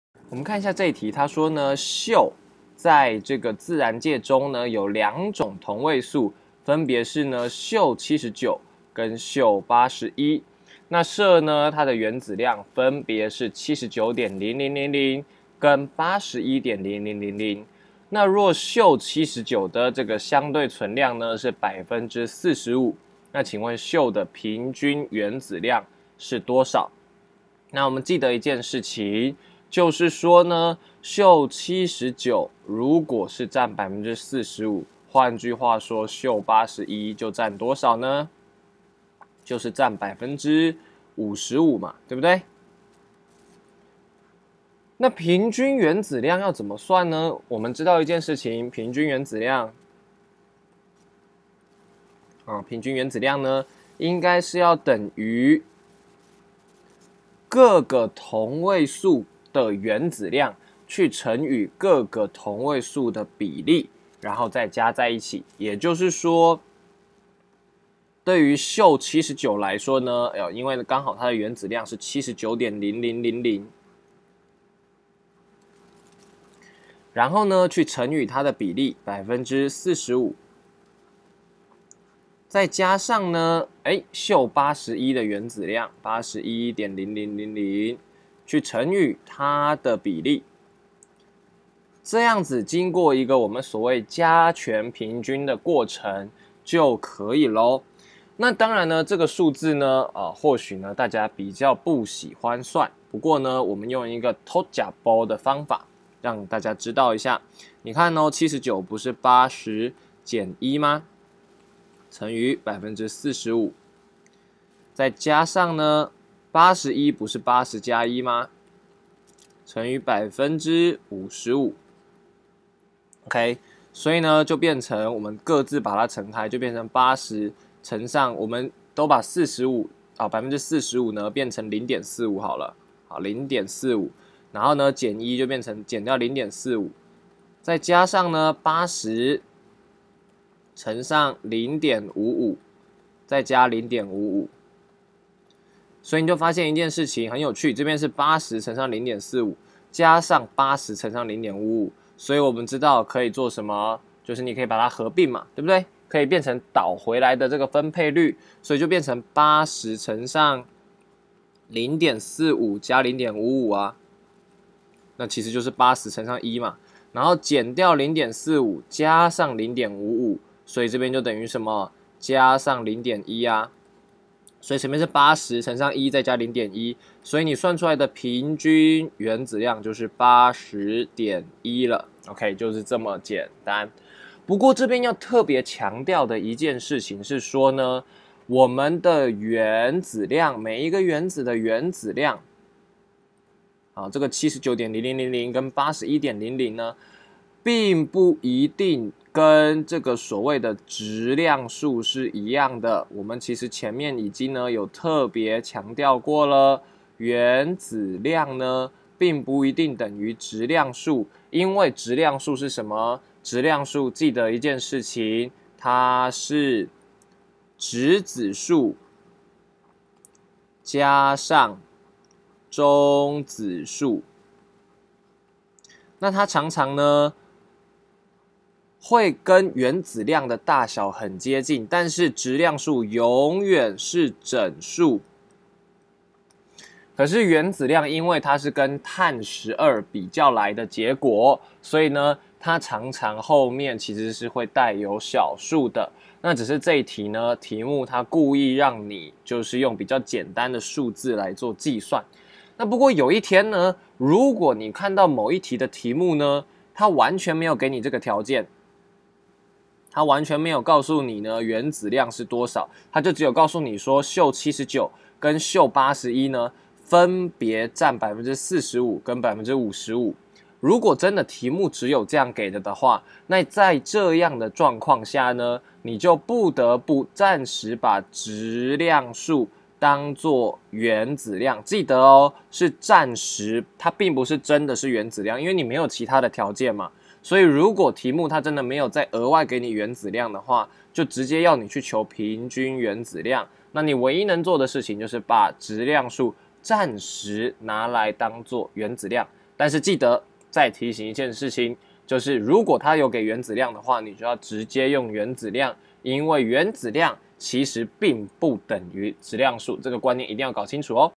2-1例題04求同位素之平均原子量
先備知識：
1.同位素的概念。

影片重點：
1.因為有同位素，所以在週期表上的原子量為「平均原子量」。
2.平均原子量為每一種同位素的原子量去乘以其所佔比例，再全部加總，是一種「加權平均的概念」。
3.質量數不等於原子量。質量數一定是整數，因為它是質子數加中子數；但原子量是與碳-12比較的結果，很有可能帶有小數。不過一般來說，質量數與原子量的大小會很接近。
4.如果題目真的沒有給同位素的「原子量」，則暫且可以把質量數當作原子量來計算。例如溴-79的原子量就直接當作是79。